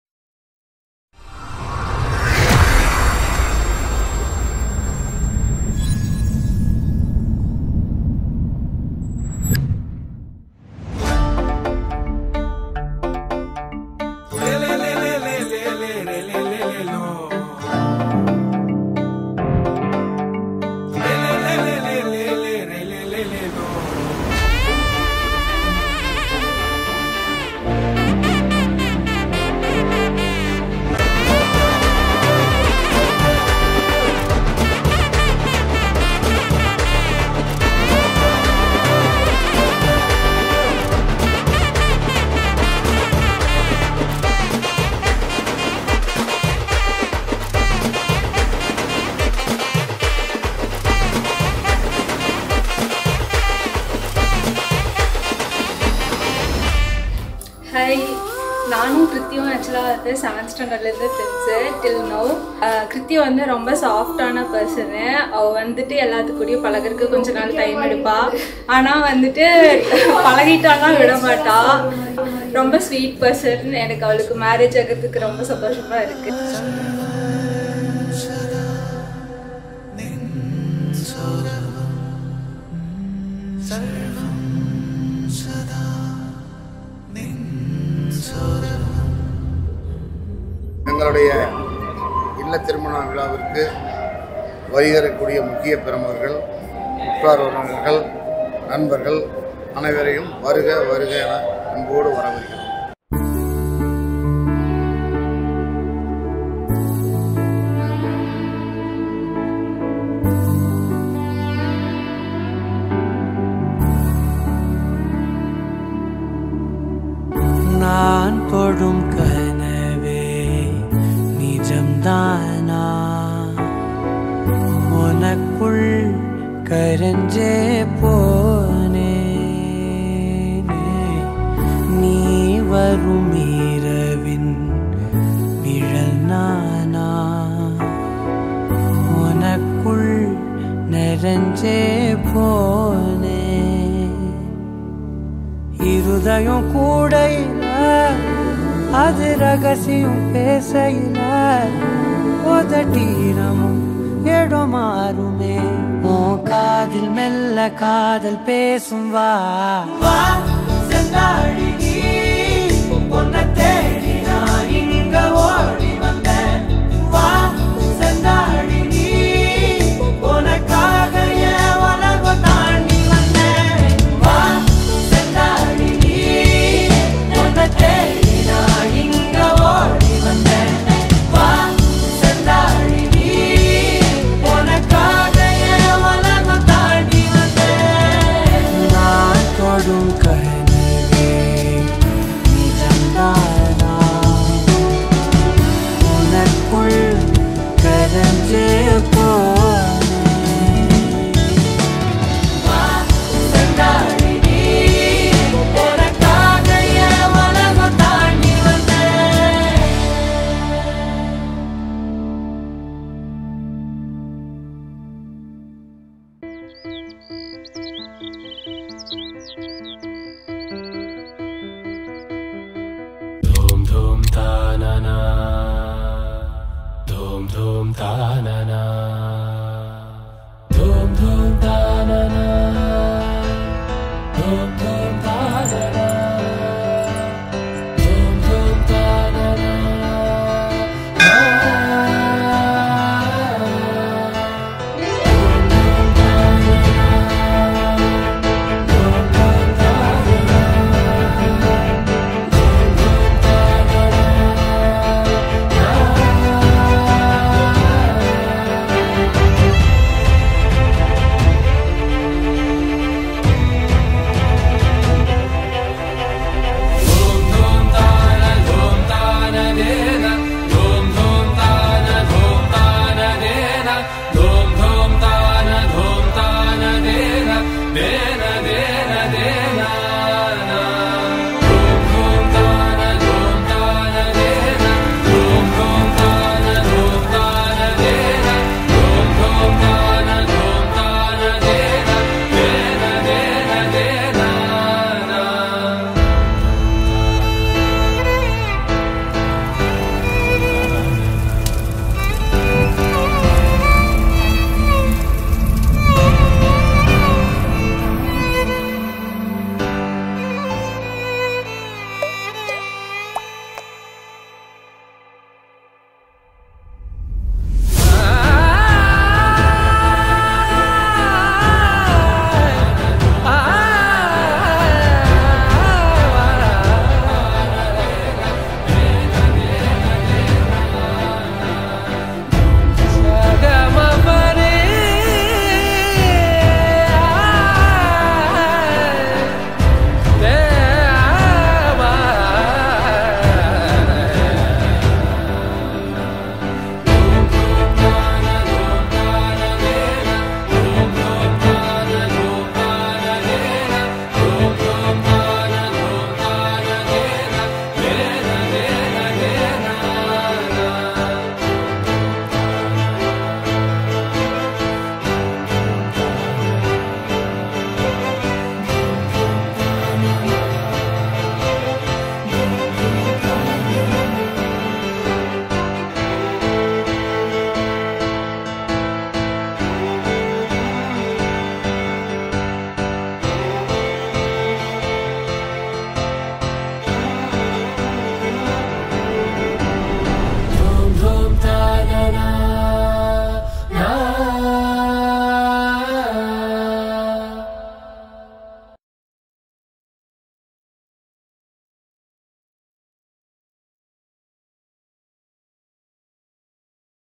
Coimbatore Grand Wedding | எட்டிமடை A.சண்முகம் Ex-MLA இல்ல திருமண விழா | SENDHUR ♥ KIRUTHIKA | 2024
Kongu Wedding Selvam Mahaal ( Eachanari ) Coimbatore.
Yuvan Photography
Pollachi
Ph ; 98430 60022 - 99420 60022